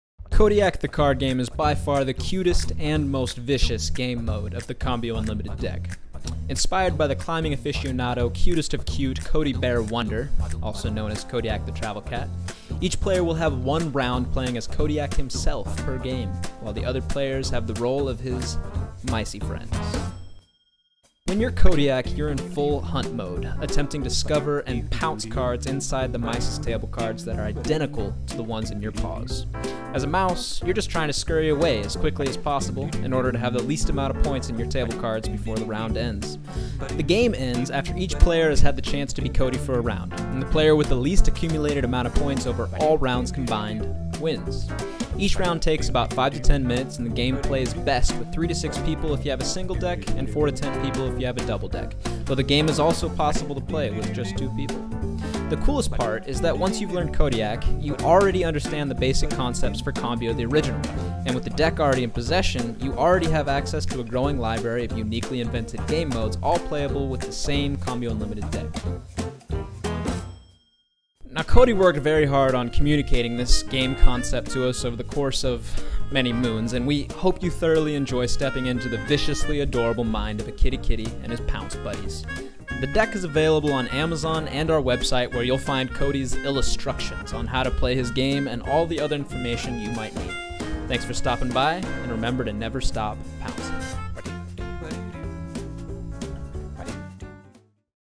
Kodiak The Card Game by Kodiak The Travel Cat
Kodiak The Card Game is by far the cutest and most vicious game mode of the Cambio Unlimited Deck.  If you'd like more information or links to buy the deck after viewing the introduction and overview you can find them here below!